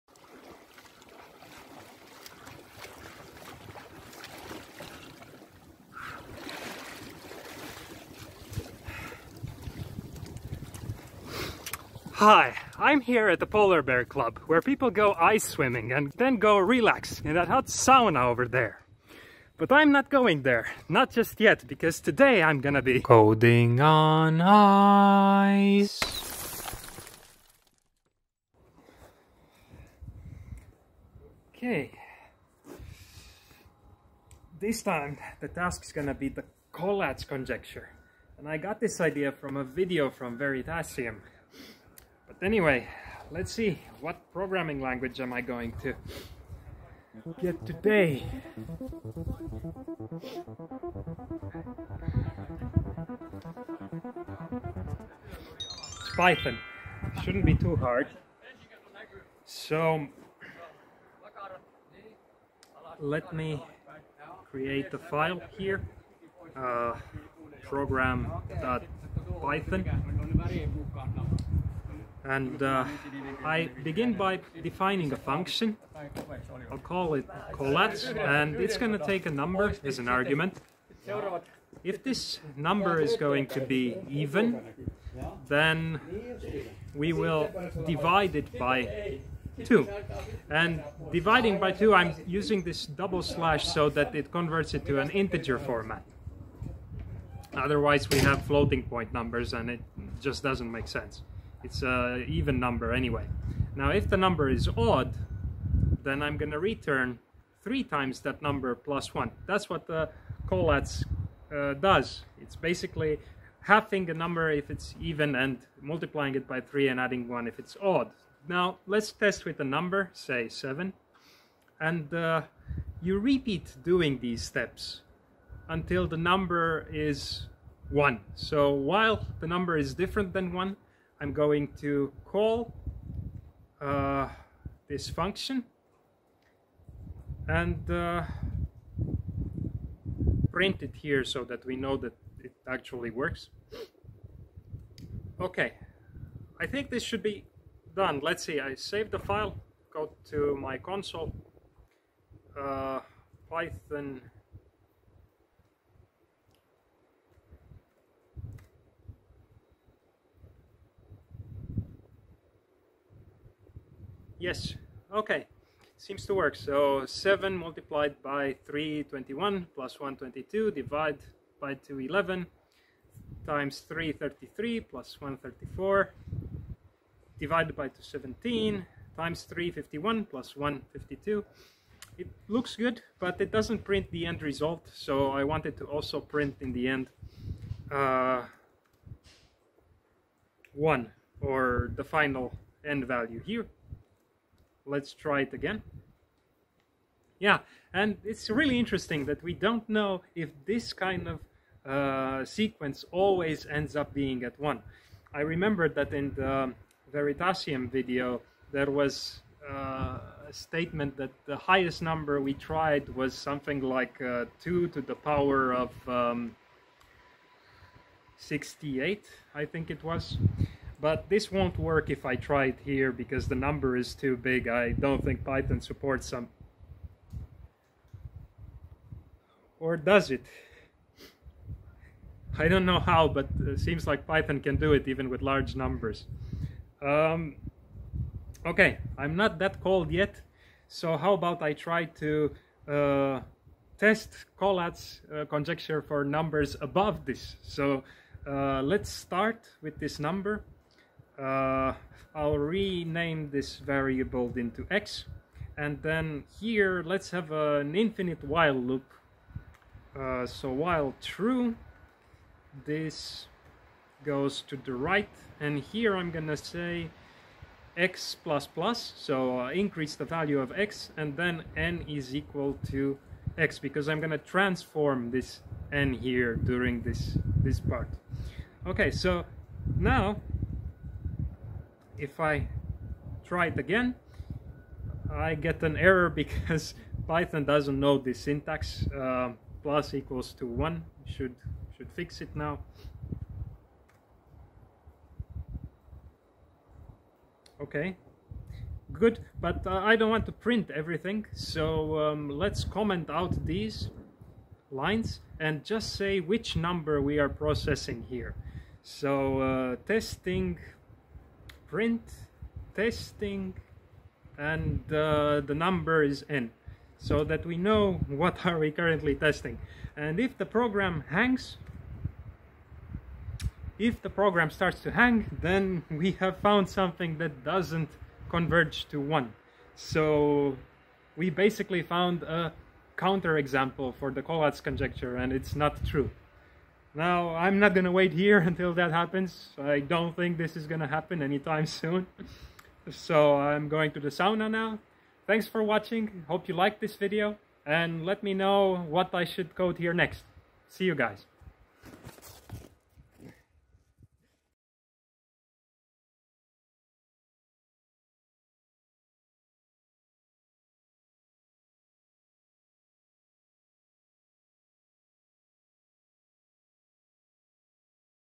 Collatz Conjecture (3n+1) in Python: Coding on Ice Ep.2
I'm at the polar bear club in Joensuu, Finland coding after an ice swimming session :-) Can I implement the collatz conjecture (3n+1) before I need to use the Sauna?